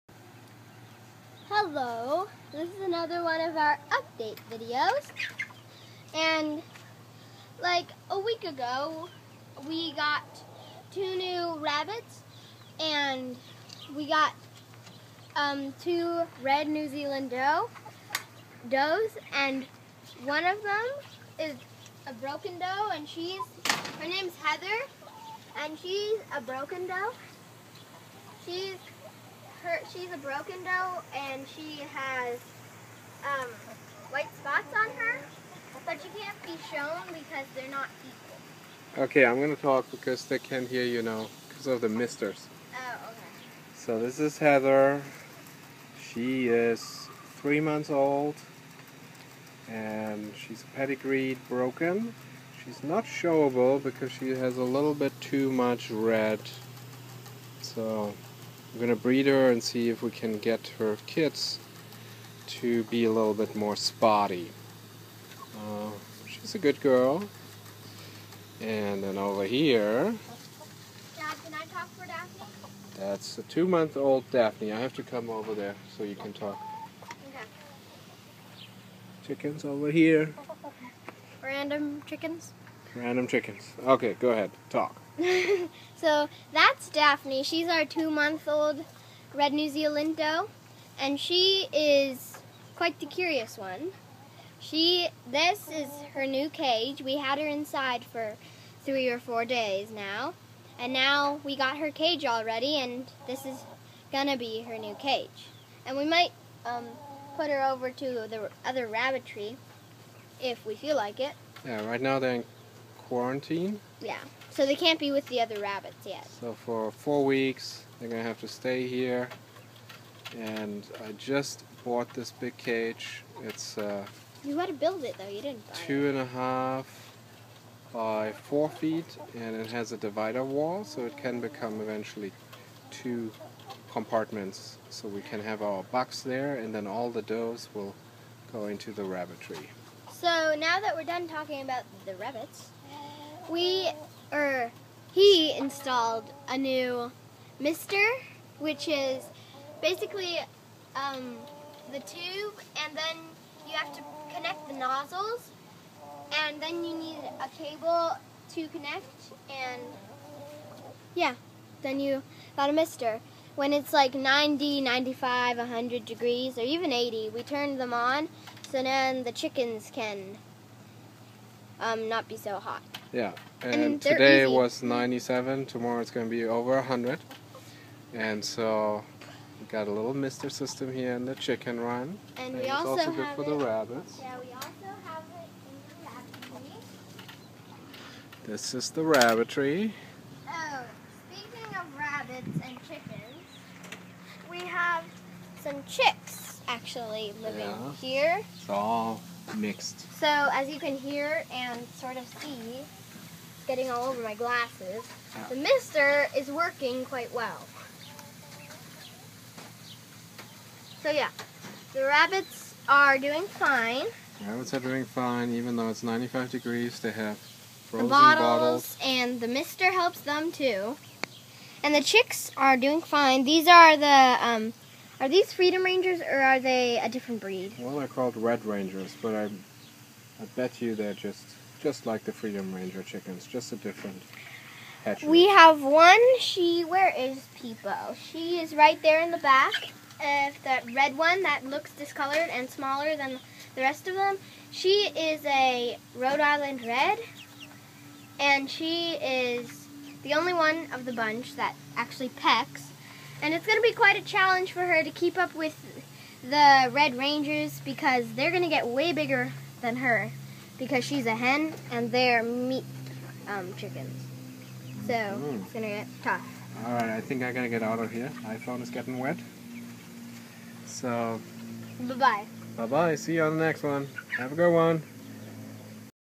Red New Zealands Rabbit Addition
We just got 2 new pedigreed rabbits. Both are Red New Zealand does, one broken one solid. We aim to bring a little color into our herd by breeding them with our White New Zealands. We are only beginning to learn about the genetics behind rabbit colors, but this is what we learned so far: To get a broken rabbit (=a rabbit with different colored spots) one of its parents has to be a broken as the broken gene is dominant (so any solid rabbit has no broken gene).

A White New Zealand is capable of giving birth to an array of different colored bunnies, as the albino gene is recessive and will only create albino when matched with another alibino gene. Whenever you breed a white with another colored rabbit it is like "lifting the veil" on the genetic background of that white rabbit and can produce black, blue, agouti style rabbits (wild looking), possibly even red. We'll do a video on rabbits genes in the near future... go for more info on Basic Rabbit Color Genetics.